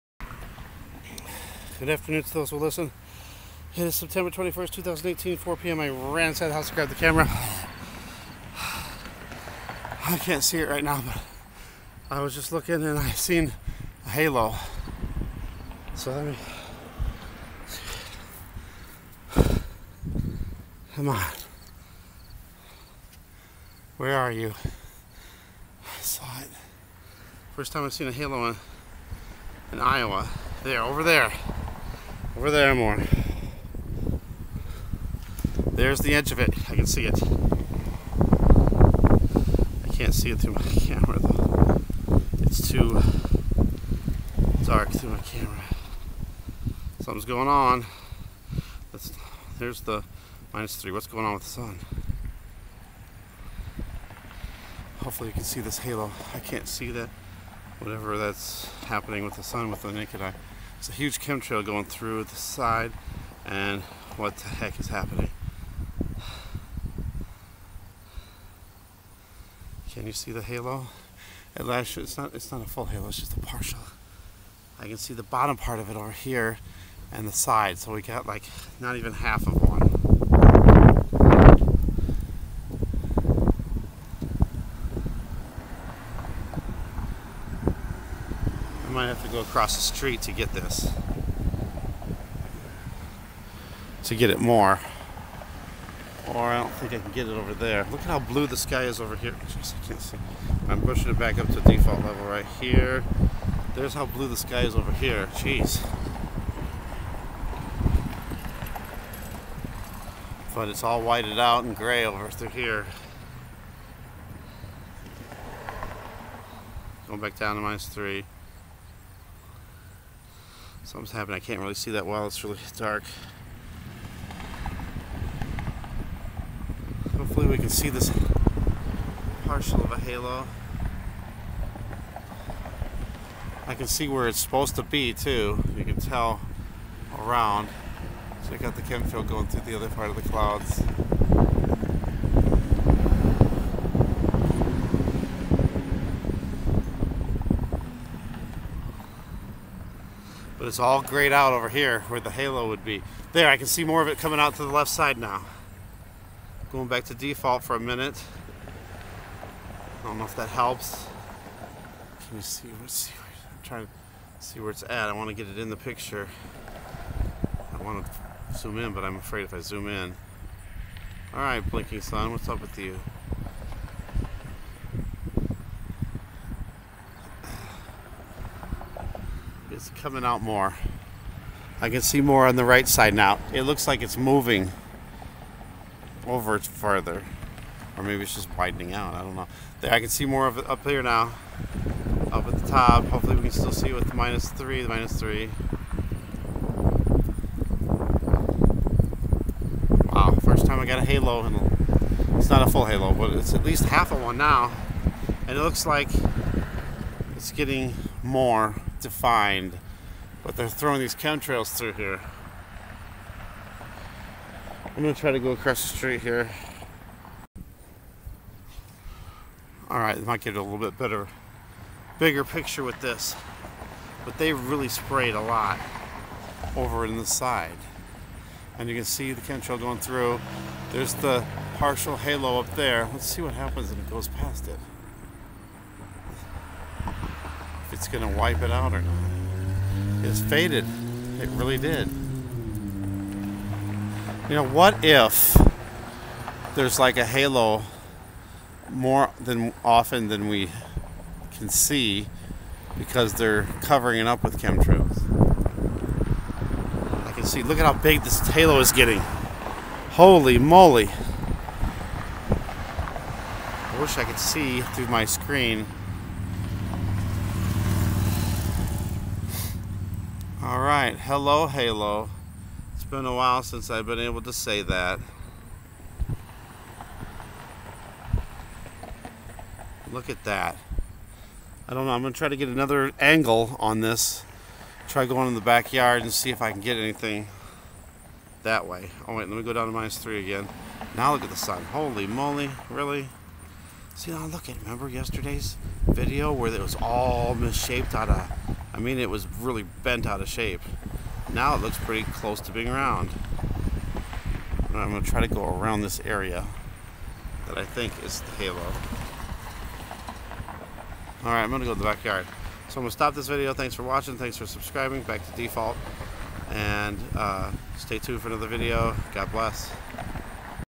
TOTAL PROOF OF SUN SIMULATOR WITH HALF HALO!!! PART 1 September 21st 2018 3:59 pm
Become a patron and receive a TTWWL coffee mug... HALF HALO WHERE I BELIEVE PLANET X SYSTEM PASSES BY LENSING SYSTEM. CLOUDS AND CHEMTRAILS PASS BEHIND THE SUN SIMULATOR YES BEHIND...PROOF OF LOCAL SUN...THEN THE TINY WHITE DISC APPEARS HUNDREDS OF TIMES SMALLER THAN EARLIER. WATCH ALL PLEASE. Please Donate If You Can...God bless...